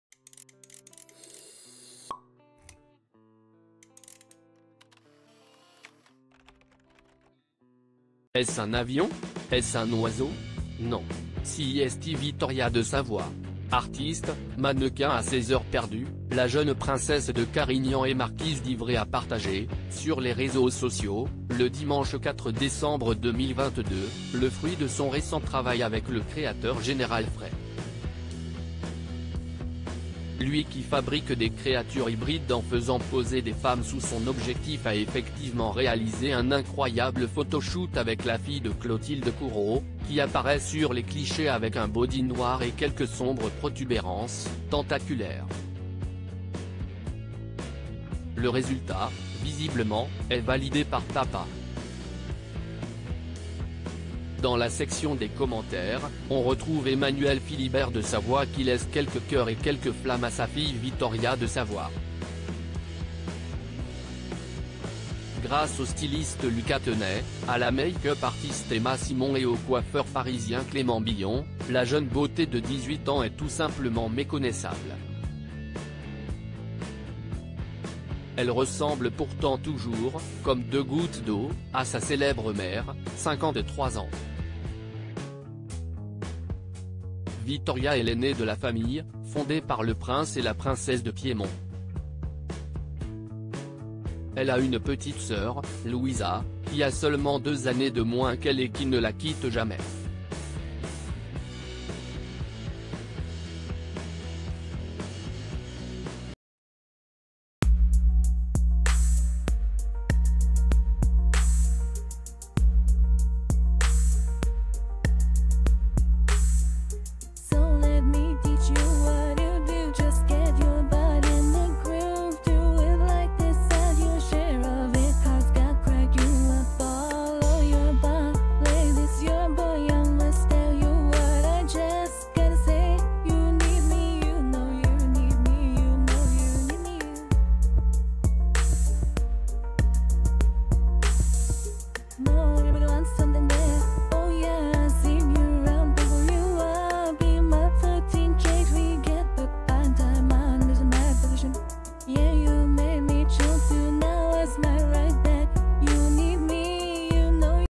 Clotilde Courau : Sa fille Vittoria méconnaissable dans un look hallucinant... papa valide quand mêm
Clotilde Courau : Sa fille Vittoria méconnaissable dans un look hallucinant... papa valide quand même !